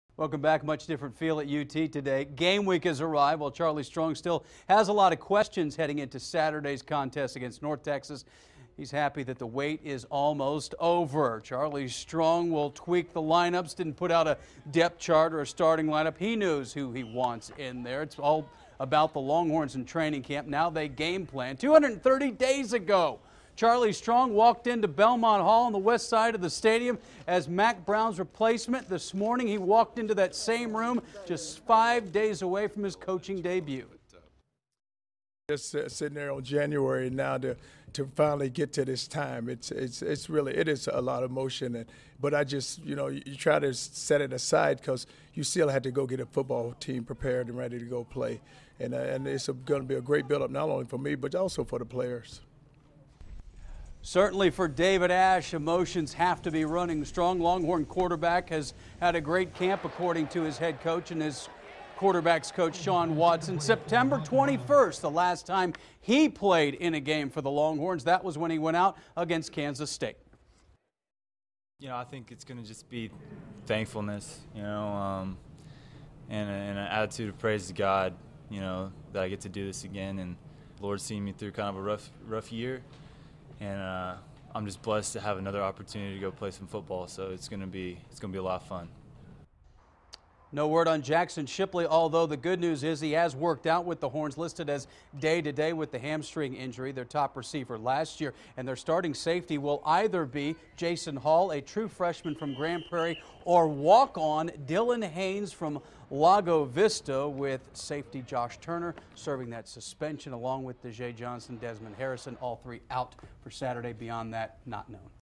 Strong excited game week is finally here
Charlie Strong was introduced as University of Texas head coach on Jan. 7. 230 days later he walked into the same room in Bellmont Hall ready to talk about game week as the Longhorns take on North Texas saturday night at Royal-Texas Memorial Stadium.